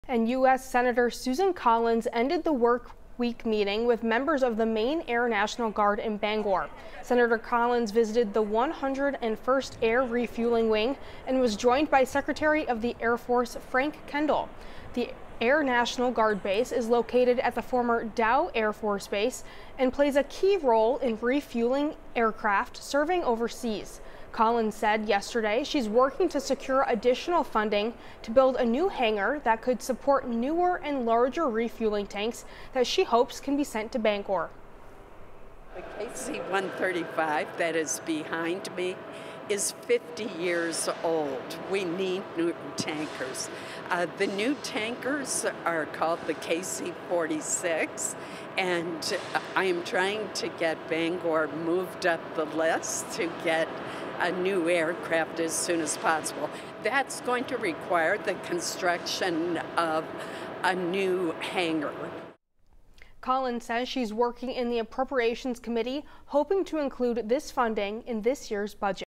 Collins meets with Maine Air National Guard members at Bangor base
Sen. Susan Collins, R-Maine, was joined by Secretary of the Air Force Frank Kendall.